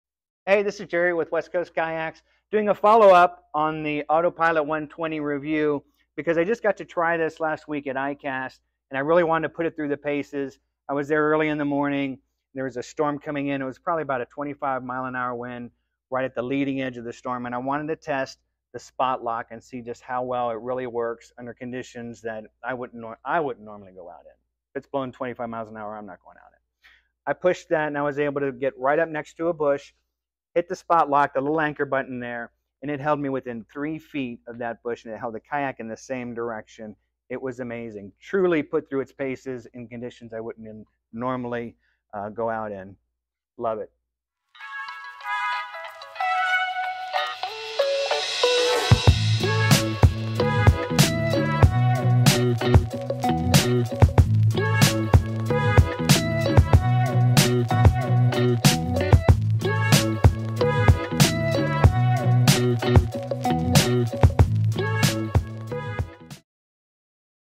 Old Town Autopilot Testimonial from ICAST
Jerry shares his experience in the 120 Autopilot by @OldTownCanoeKayak!
Is this the right kayak for you?? Come on by @WestCoastKayaksFL and take a look for yourself!